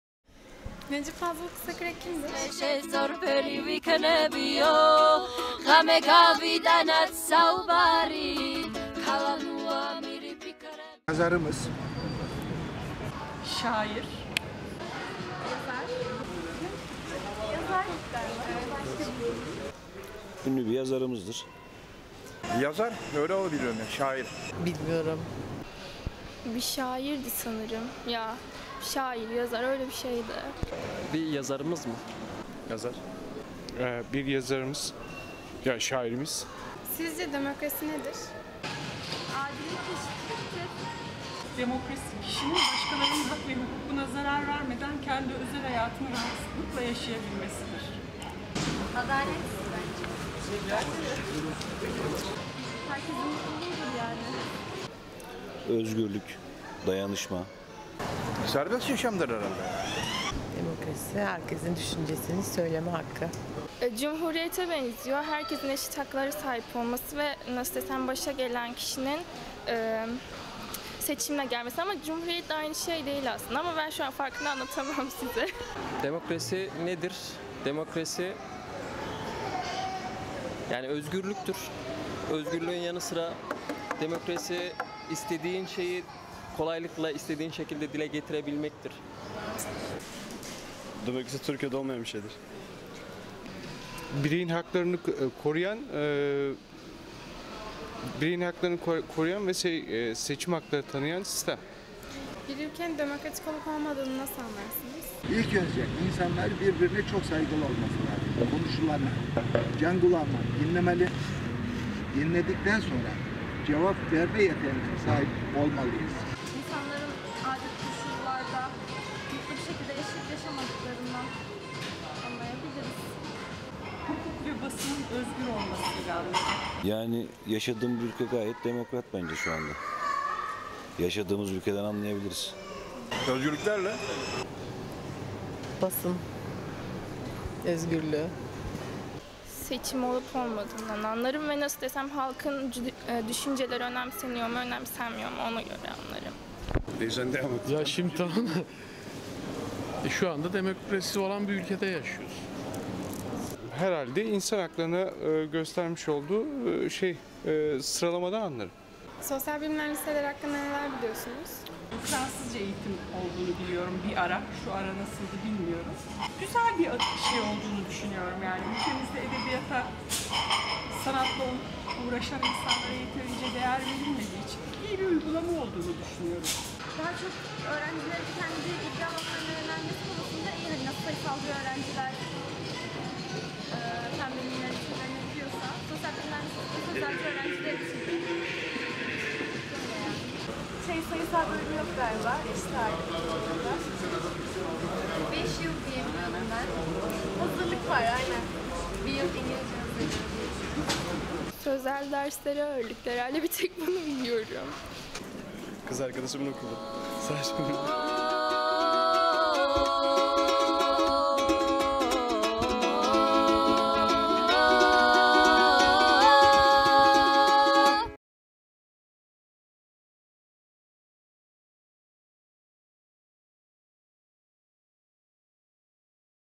Sokak Röportajları | Necip Fazıl Kısakürek, Sosyal Bilimler, Demokrasi
Salihli'de sokakta, parkta ve alışveriş merkezinde karşılaştığımız vatandaşlara Necip Fazıl Kısakürek, Sosyal Bilimler Liseleri ve Demokrasi hakkında birkaç soru sorduk.